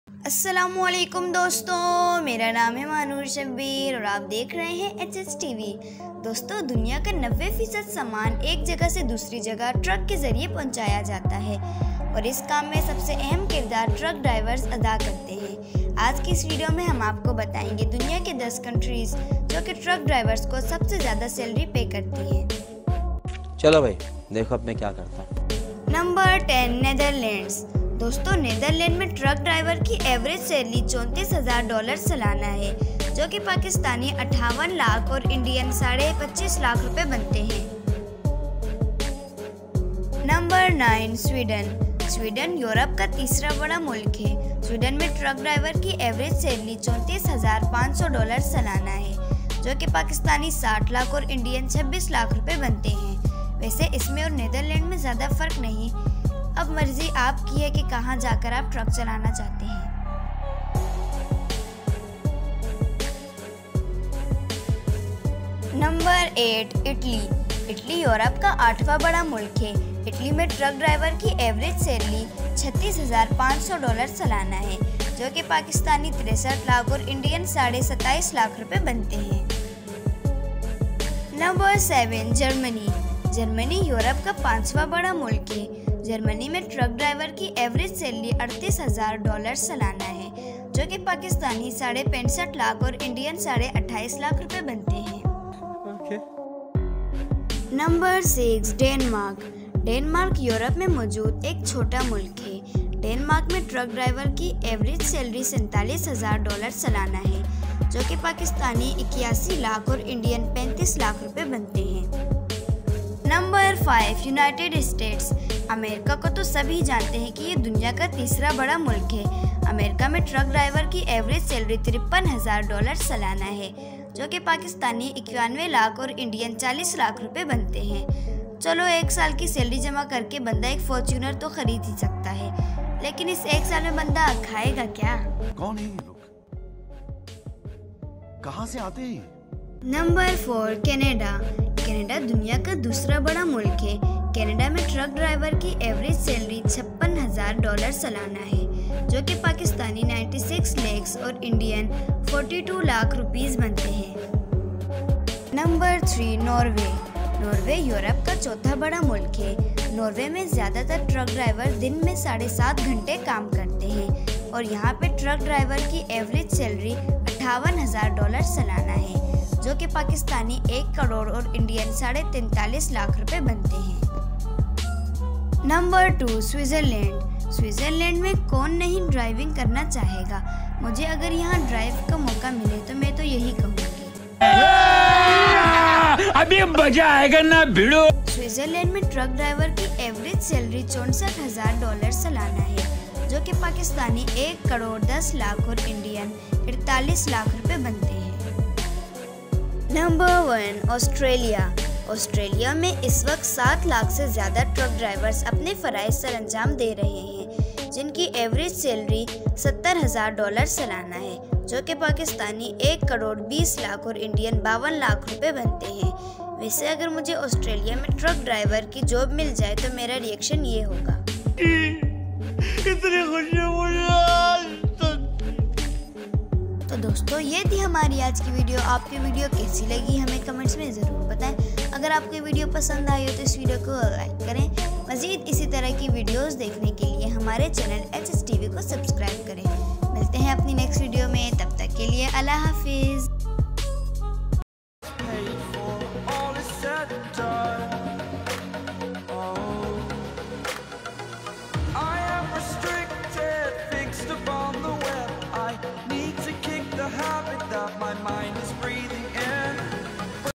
10 countries with the highest truck driver salaries in the world | HH TV
10 countries with the highest truck driver salaries in the world .